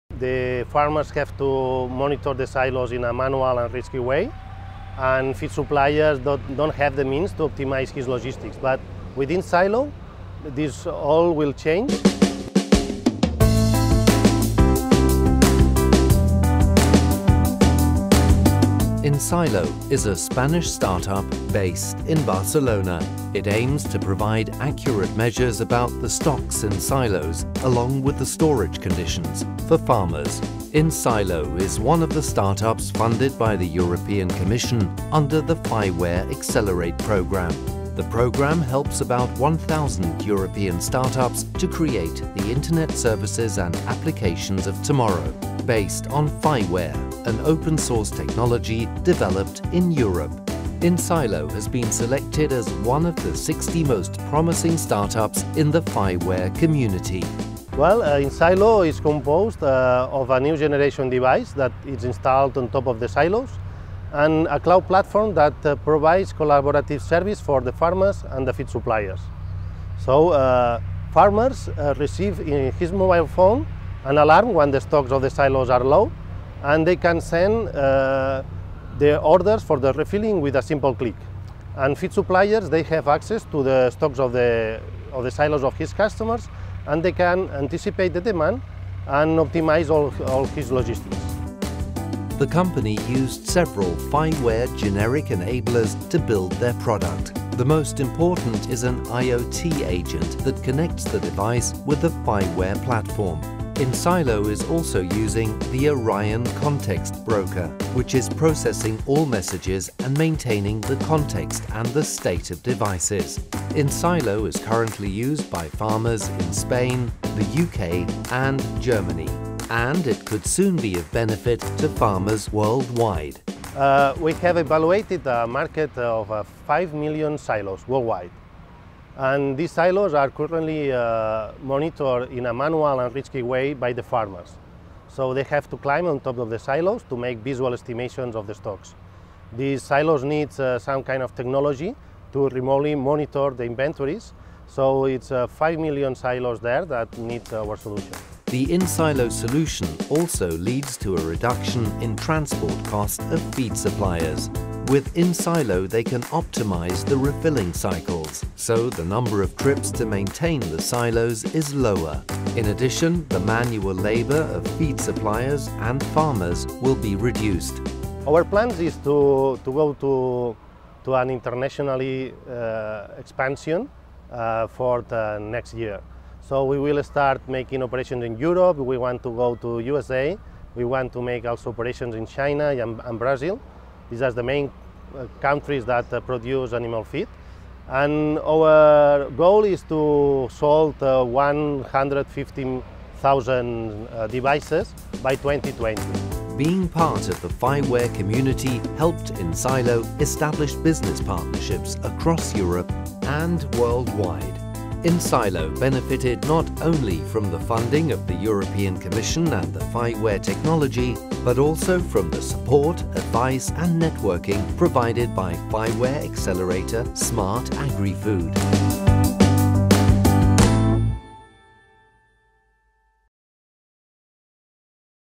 InSylo: Smart Monitoring of Silos - FIWARE Business Success Story
InSylo is a Spanish startup based in Barcelona. It aims to provide accurate measures about the stocks in silos, along with the storage conditions, for farmers. InSylo is one of the startups funded by the European Commission under the FIWARE Accelerate programme. The programme helps about 1,000 European startups to create the Internet services and applications of tomorrow based on FIWARE, an Open Source technology developed in Europe. InSylo has been selected as one of the 60 most promising startups in the FIWARE community.